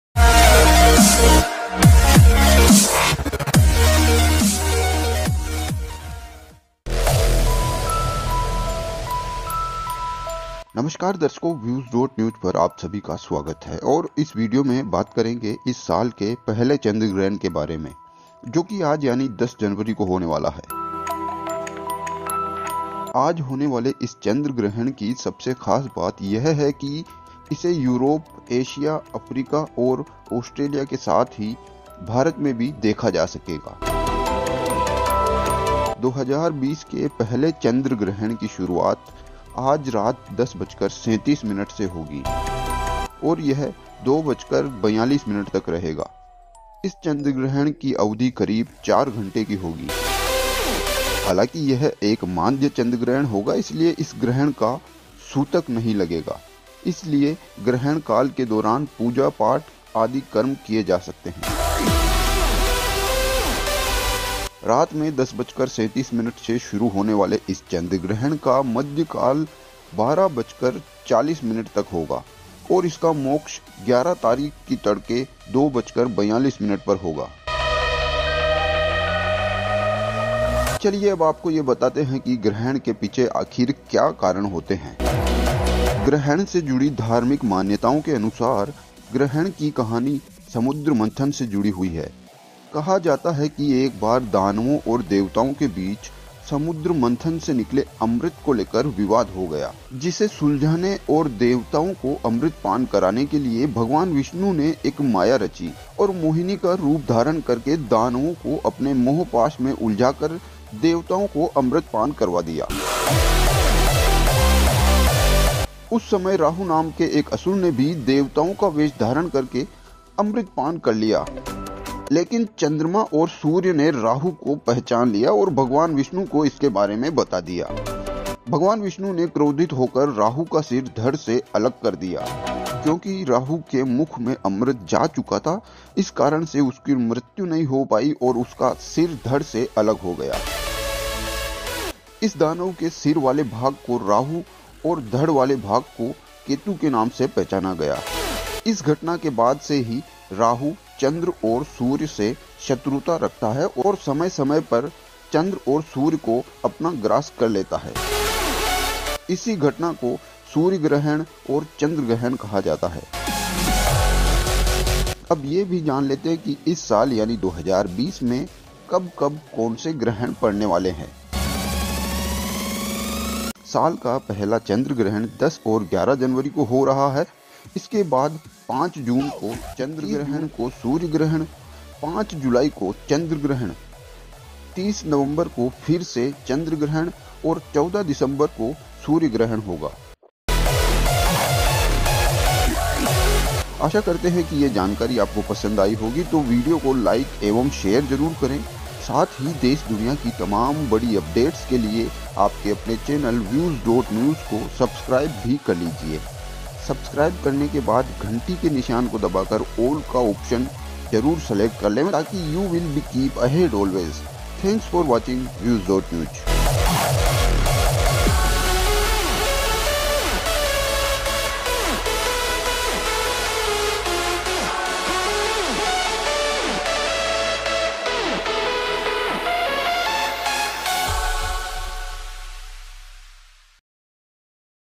जानिए, आखिर क्यों होते हैं चंद्रग्रहण और सूर्यग्रहण । The First Lunar Eclipse of 2020 Being Tonight
Know all about LunarEclipse or ChandraGrahan January 10, 2020.

Lunar eclipse is the time when the earth blocks the light of the sun from reaching the moon. There are three types of LunarEclipse2020 - total, partial and penumbral. The January 10 penumbral lunar eclipse is one of the four lunar eclipses that will take place in the year. The first lunar eclipse of the year 2020 will start on January 10 at 10:39 pm.

This lunar eclipse ChandraGrahan2020 will be penumbral, which means the Earth will block some of the sunlight from reaching the moon and only the outer shadow (the penumbra) will fall on the moon. The January 10 lunar eclipse will coincide with the full Moon.